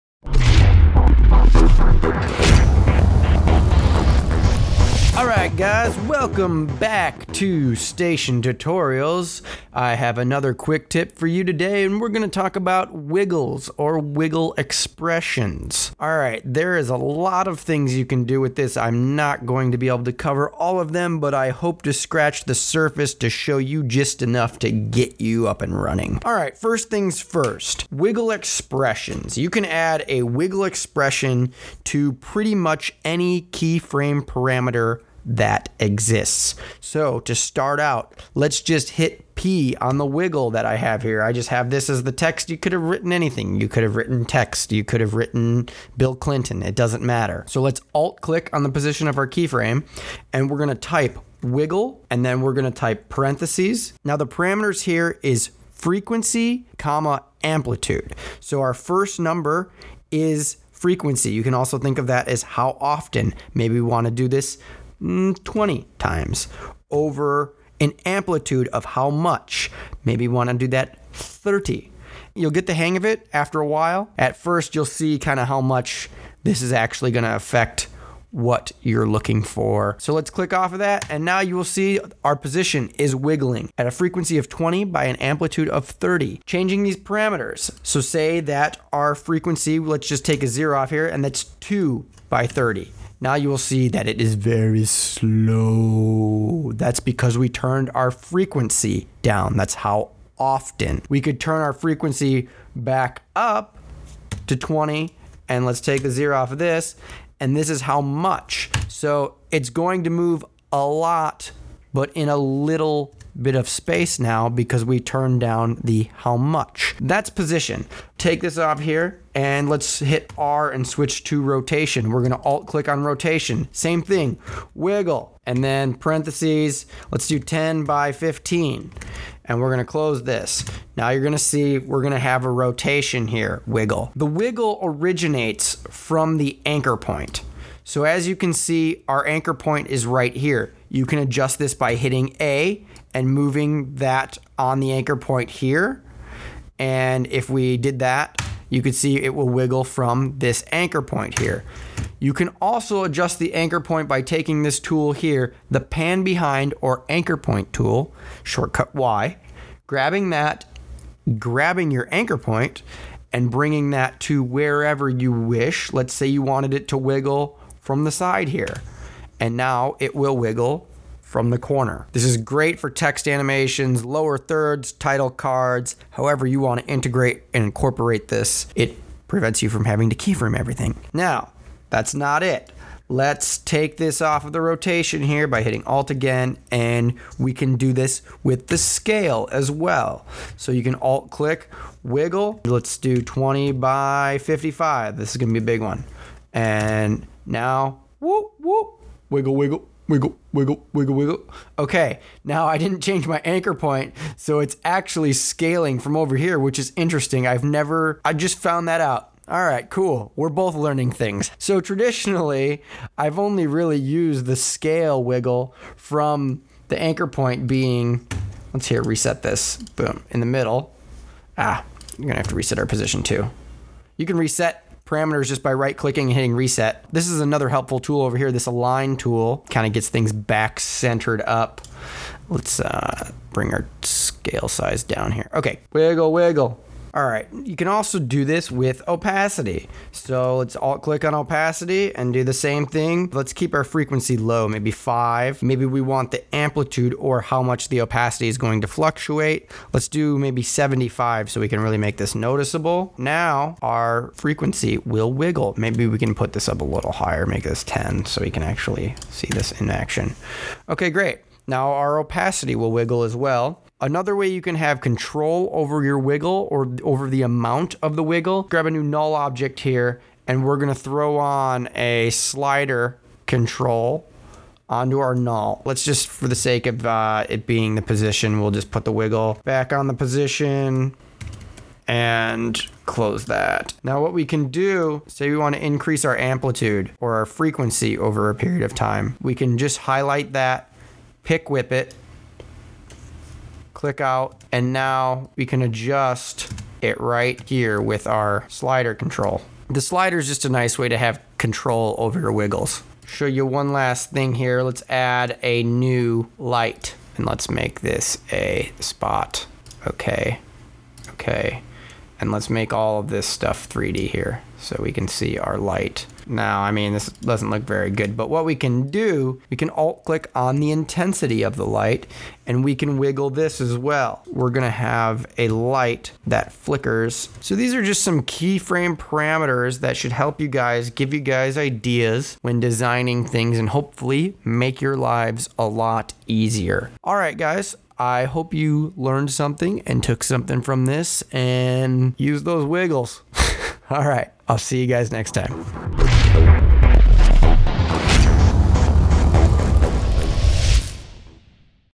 Wiggle Expressions in After Effects
After Effects Quick Tip:
How to use wiggle expressions to automate keyframing.